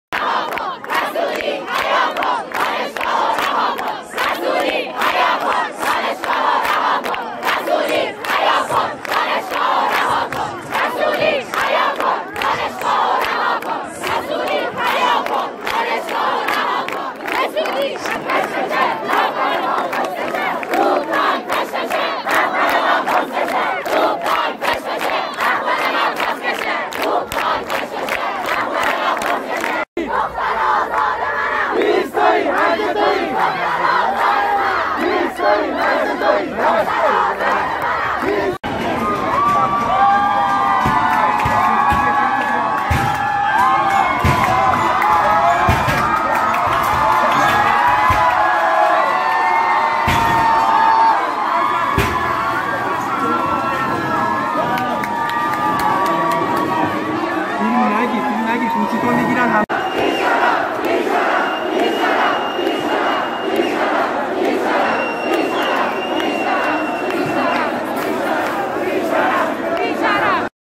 مهدی رسولی مداح جیره خور خامنه ای اومده بود دانشگاه زنجان نماز بخونه. رسولی حیا کن دانشگاه رو رها کن
امروز دانشجویان دانشگاه زنجان هم بپاخاستند و دست به اعتراض گسترده زدند
کسی که فیلم گرفته نوشته:  مهدی رسولی مداح جیره خور خامنه ای اومده بود دانشگاه زنجان نماز بخونه
همه جمع شدن شعار دادن:
 رسولی حیا کن دانشگاه رو رها کن
هیز تویی هرزه تویی . زن آزاده منم
رسولی برو گمشو